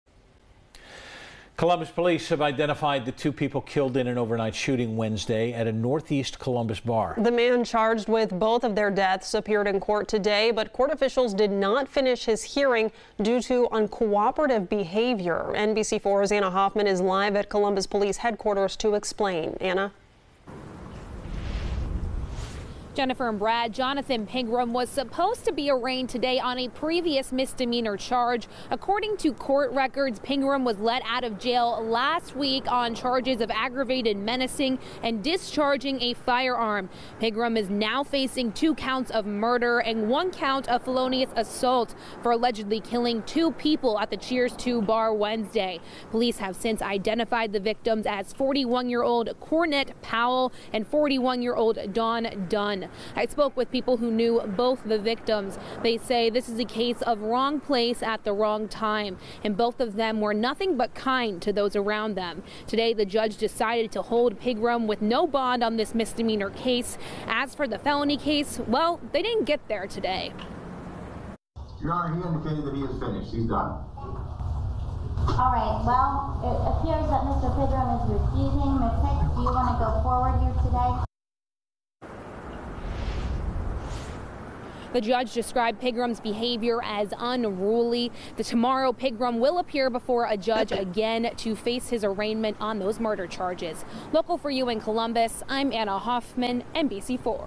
Cheers Too double murder suspect faces judge
Police have identified the two people shot and killed at a northeast Columbus bar, as the suspect appeared in court on Thursday.